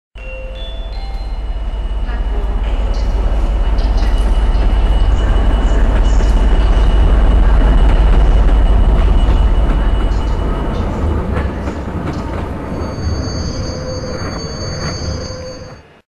66709 arriving at Ash Grove [Trago Mills Model Railway]
Medite livery class 66 no. 66709 'Joseph Arnold Davies' stops at Ash Grove station with a train of hoppers on the Trago Mills Model Railway